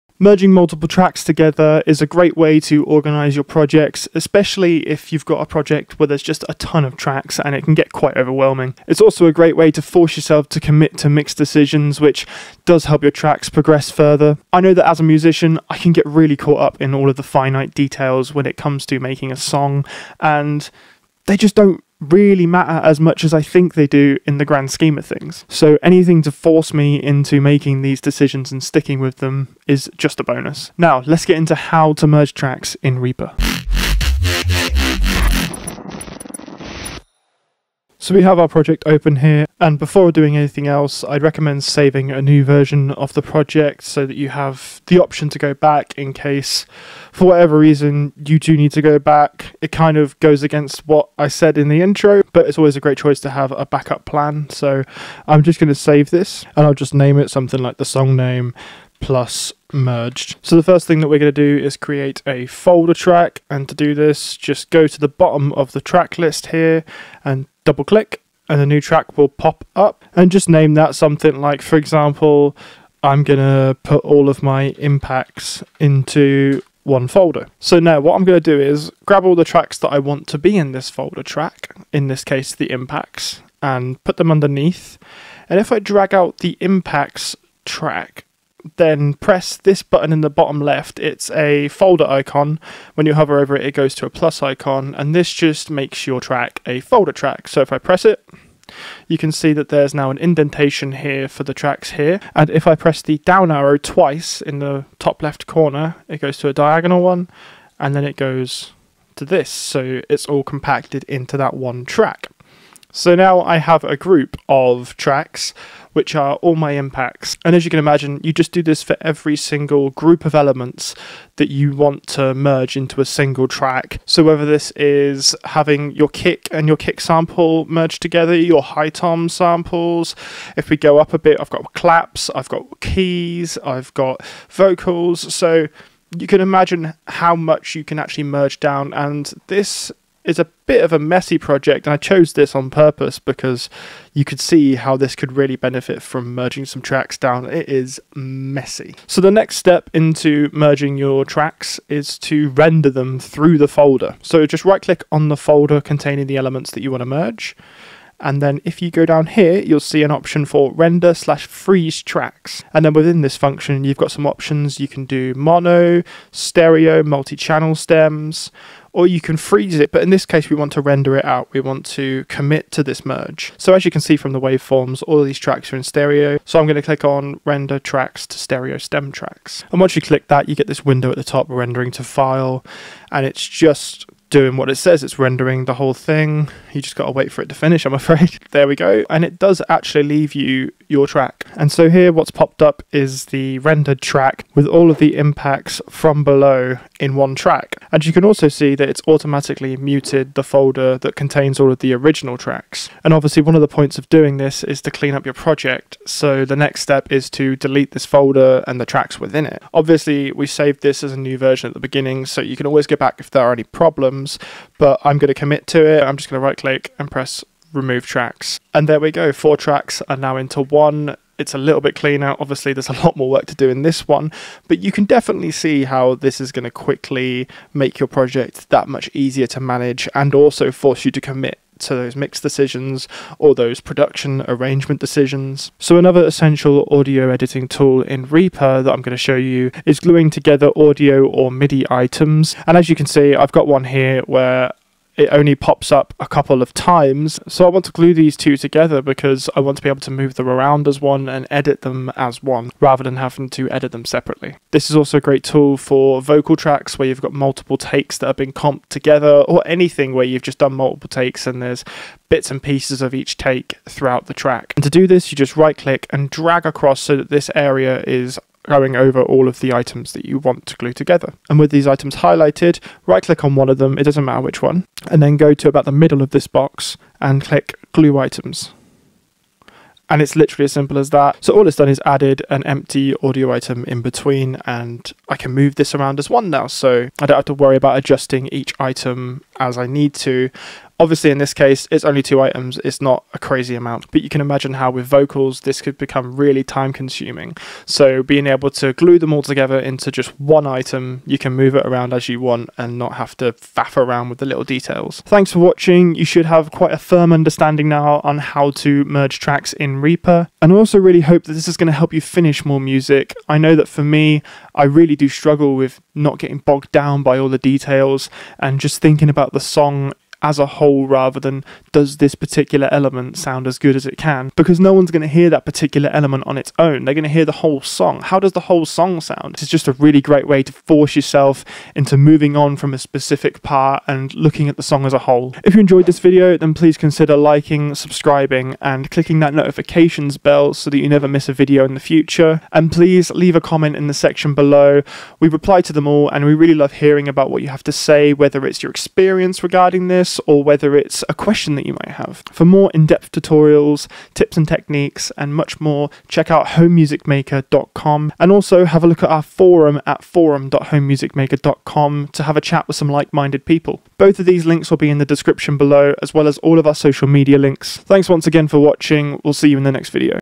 Merge Tracks in REAPER Like a Pro! (Simple Step-By-Step Guide)
Ready to declutter your REAPER workspace and merge tracks like a pro?

In this video, Jack's got you covered! He’ll dive into saving new versions of your project for safety, creating folder tracks to keep things neat, and even rendering those tracks for a final touch. Jack will also show you how to delete the original tracks you don't need and glue items together for that seamless sound.

Trust us: by the end of this video, your REAPER projects will be as clean as a whistle!

⏱️TIMESTAMPS⏱️
00:00 - Intro
00:38 - Saving A New Version
01:01 - Create A Folder Track
01:18 - Putting Tracks Into The Folder Track
02:30 - Render The Folder Track
03:24 - Delete The Original Tracks
03:56 - Gluing Together Items
05:04 - Outro

HELPFUL LINKS